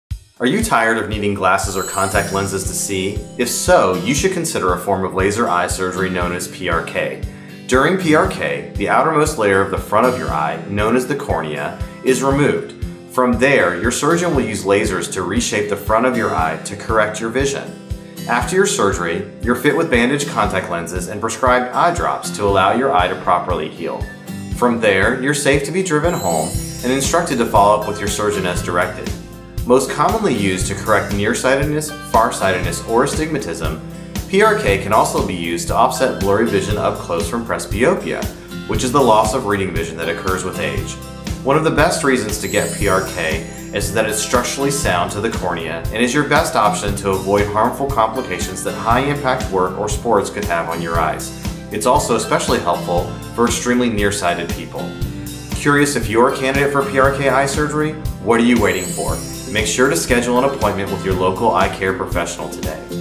PRK Eye Surgery | What Are The REAL Benefits of PRK?
Dr. Vipul Shah discusses the benefits of getting PRK eye surgery.